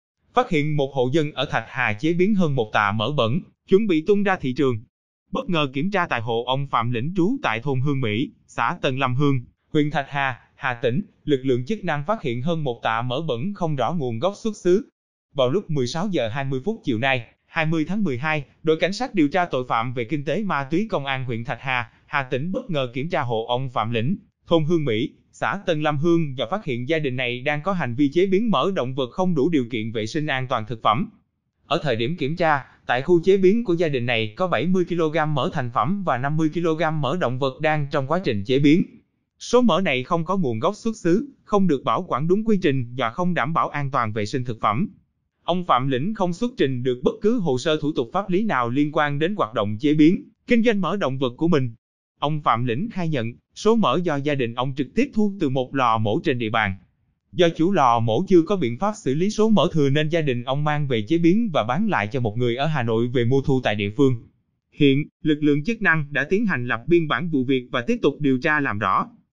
Phát hiện 1 hộ dân ở Thạch Hà chế biến hơn 1 tạ mỡ “bẩn” chuẩn bị tung ra thị trường
Bất ngờ kiểm tra tại hộ ông Phạm Lĩnh (trú tại thôn Hương Mỹ, xã Tân Lâm Hương, huyện Thạch Hà, Hà Tĩnh), lực lượng chức năng phát hiện hơn 1 tạ mỡ “bẩn” không rõ nguồn gốc xuất xứ.